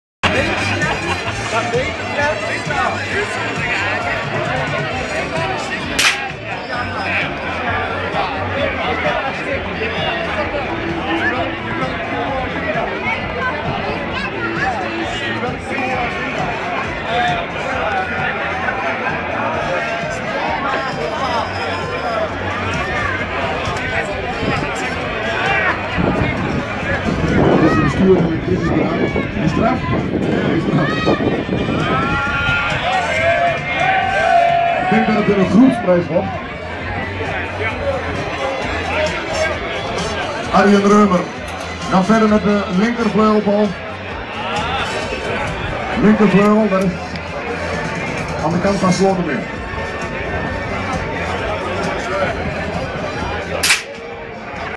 Polse Kermis 2009 Vogelschieten Ulft
Het vogelschieten / koningschieten op de maandagmorgen van de Polse Kermis 2009 in Ulft bij Schuttersgilde Sint Joris.